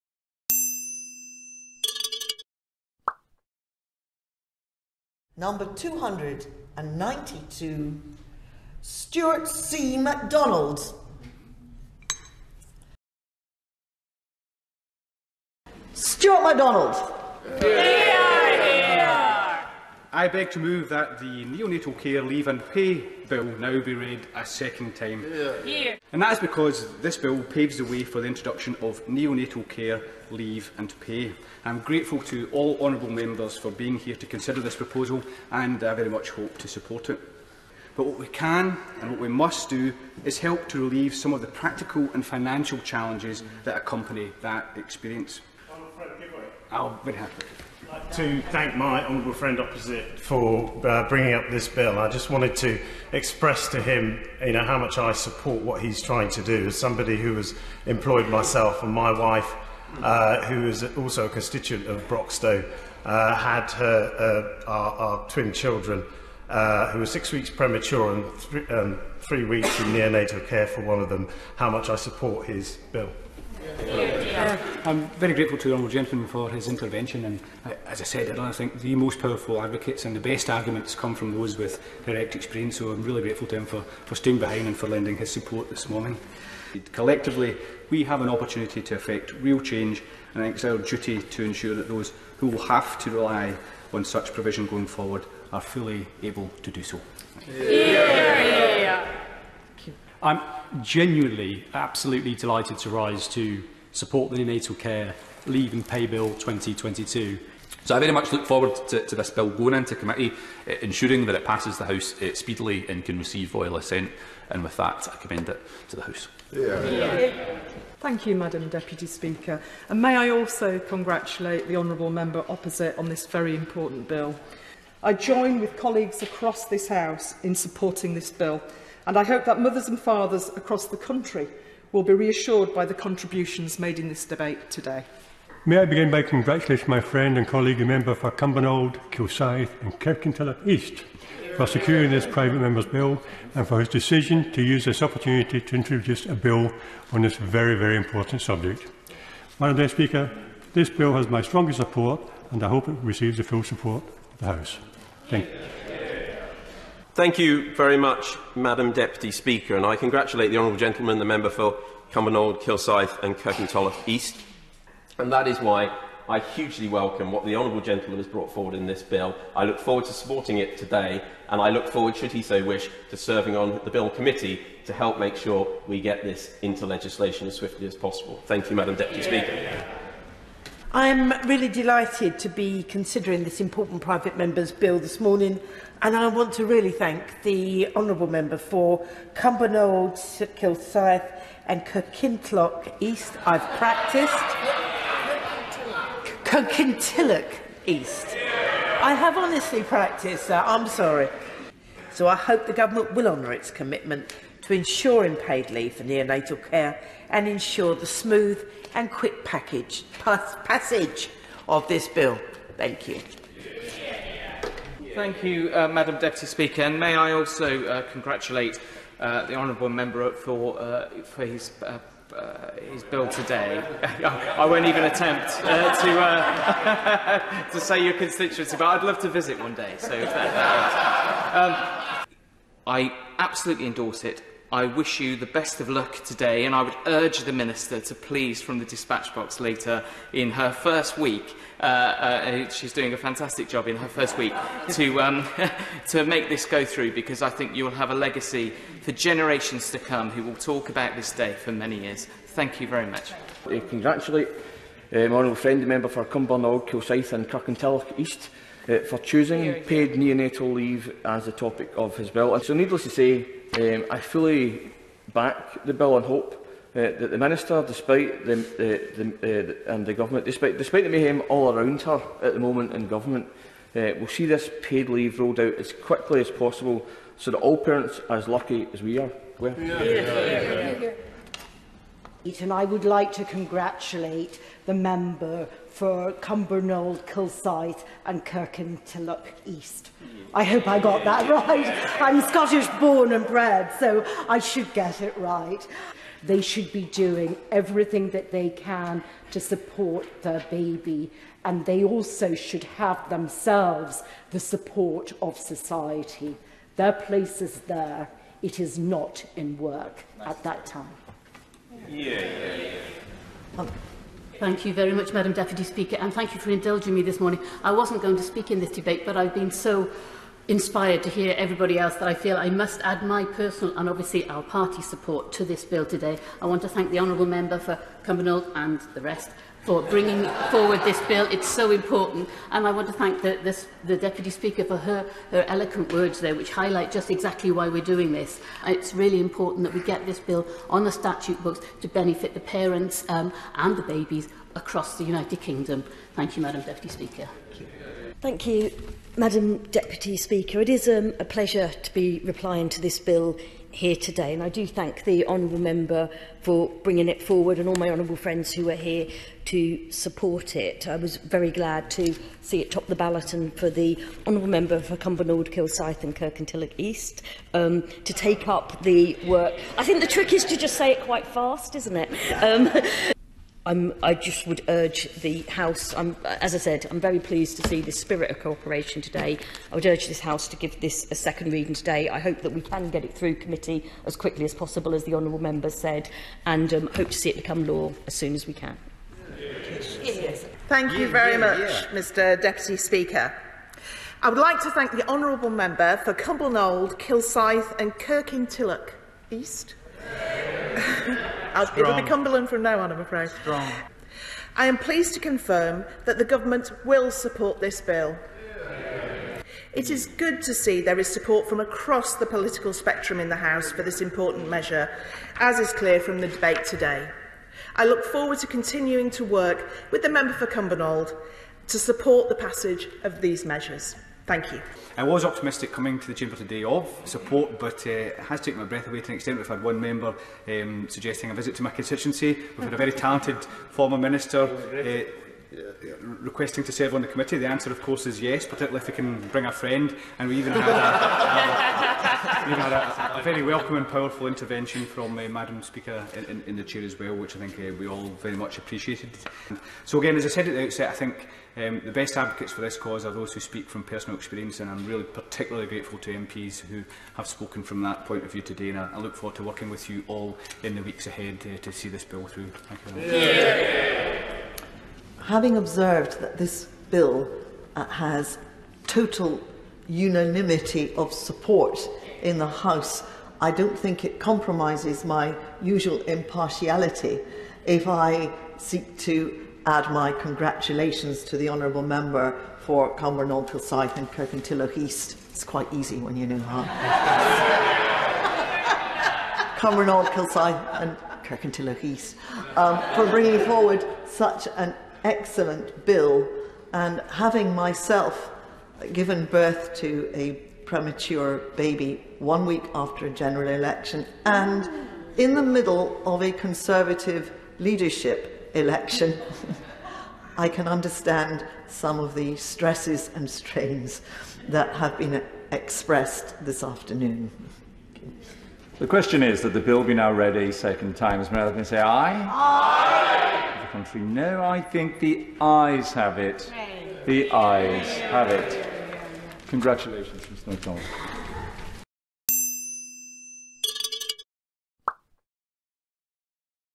Parliament unites (beautiful moment) - hilarious pronunciation problem
Private members bill for neonatal care unites the entire house of commons in what can only be described as a very touching moment when all parties forgot their differences. Enhanced by the hilarious antics of some MPs attempting to pronounce "Kirkintilloch East". Even both deputy speakers of the house weigh in with her support for this rare and beautiful moment in the houses of parliament.
Humans are crap. almost always.

00:00 Intro
00:20 Stuart MacDonald proposes bill
00:50 Darren Henry support
01:40 Luke Hall support
01:50 David Linden support
01:58 Jill Mortimer support
02:15 Allan Dorans
02:37 Edward Argar support
03:00 Lyn Brown support with Kirkintilloch
03:37 Dean Russell support with Kirkintilloch
04:13 Gavin Newlands support
04:43 Felicity Buchan support with Kirkintilloch
05:14 Ruth Jones support with Kirkintilloch
05:50 Kerry McCarthy support with Kirkintilloch
06:30 Jane Hunt support with Kirkintilloch
07:05 Stuart MacDonald sums up
07:45 Eleanor Laing touching comments
08:39 Nigel Evans passes bill

Clips courtesy of Westminster parliament channel & Channel 4
Youtube common use license